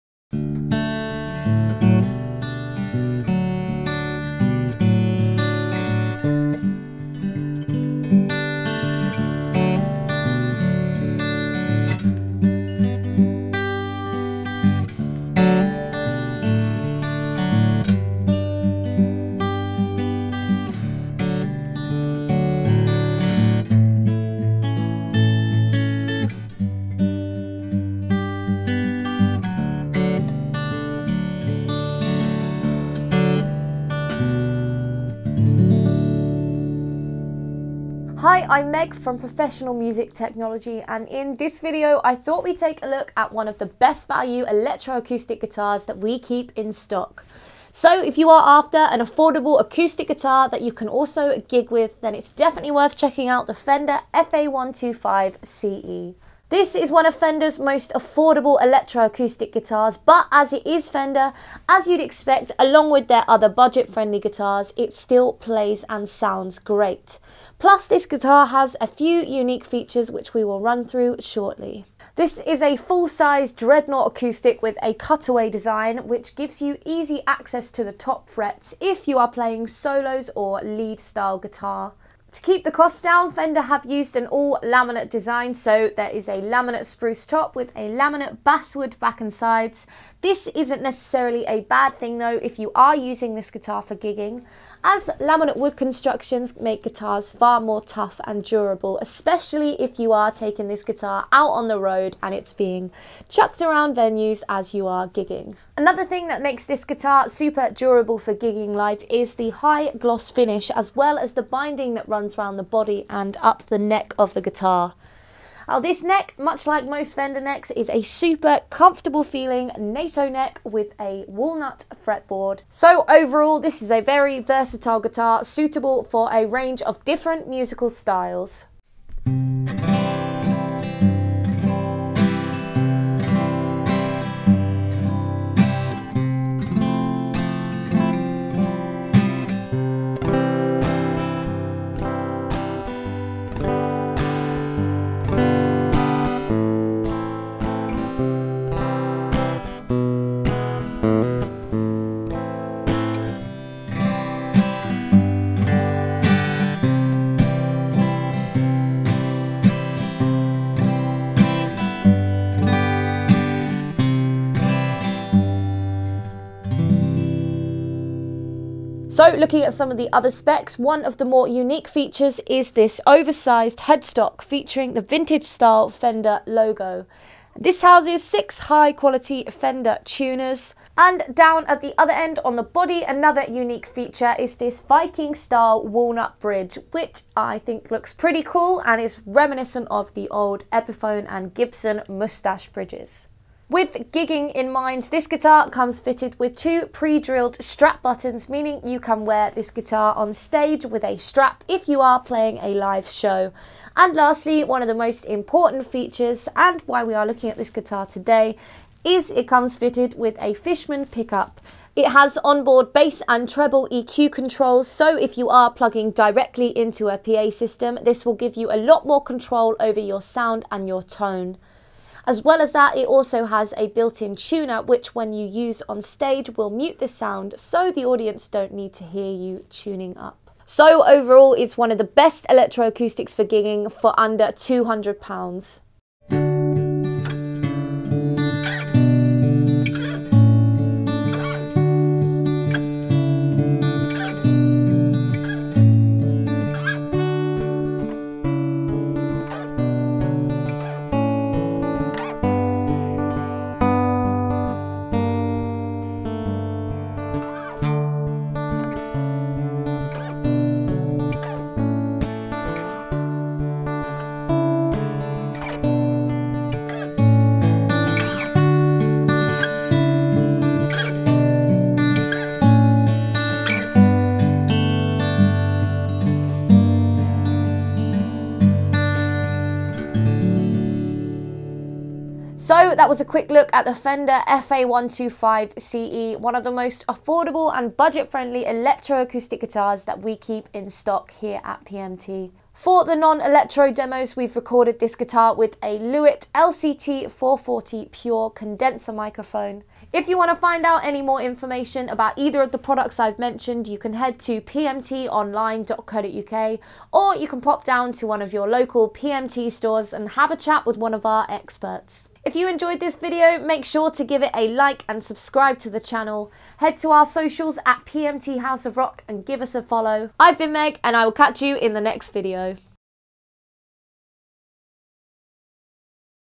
Fender FA-125CE - Best Cheap Electro-Acoustic Guitar?
Meg takes a closer look at the Fender FA-125CE, a fantastic value, gig-ready electro acoustic dreadnought.  Find out more at PMT Online...

00:00 Fender FA-125CE Demo #1
00:37 What is the Fender FA-125CE?
01:21 Body & Neck Specifications
02:08 Demo #2
02:48 Hardware & Electronics
03:55 Demo #3
04:42 Where To Buy A Fender FA-125CE

Other products used in this video...

If it's a full-sized Dreadnought electro acoustic guitar that you want, with great tone, effortless playability, and an awesome asking price, then look no further.  The Fender FA-125CE Dreadnought is a prime example of just how good Fender's more-affordable acoustic guitars can be.  Featuring a quality laminate construction, Walnut fingerboard, smooth Nato neck, and Fishman electronics, this is a guitar that was made to perform.

As any acoustic player of the last century will admit, there's nothing quite like the sound of a full-sized Dreadnought acoustic guitar.  With their body sizes resulting in a lively, dynamic, and resonant sound, the Dreadnought has been the guitar of choice for countless acoustic players for the best part of a century.

This particular guitar - the Fender FA-125CE - might well follow some of the traditional design characteristics of a Dreadnought, Fender have opted to include some modern refinements into the mix.  These include a soft lower-bought cutaway, which grants you easy access to the guitar's upper frets.

In addition to this there's a Fishman electronics system, which allows you to plug directly into an amp or mixing desking, making it ideal for both live performances and studio work.  There's also a modern Fender 3+3 headstock and Viking bridge, that work together to create a superb-sounding guitar.  And finally, the Nato neck has been designed to appeal to beginners and developing players, offering a smooth, easy to play feel.

Fender FA-125CE Dreadnought Main Features:

- Affordable Dreadnought-style electro acoustic guitar
- All-laminate construction
- Modern 3+3 headstock
- Viking bridge
- Nato set-neck
- Fishman preamp system
- Ideal for relative beginners and players on a budget